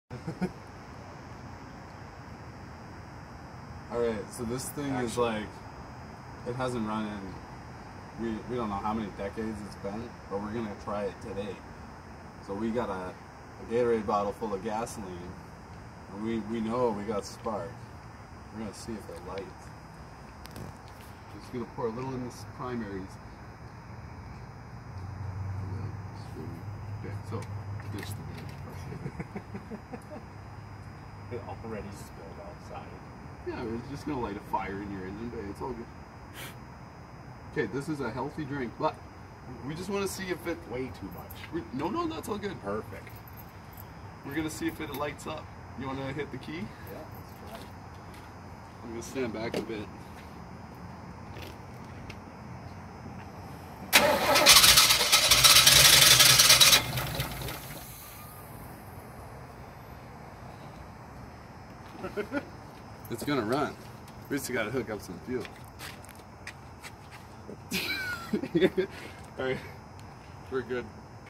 Starting Lucy the 1972 C10 for the first time in decades (Part 1)
My brother-in-law and I starting my truck that hasn't run in over 20 years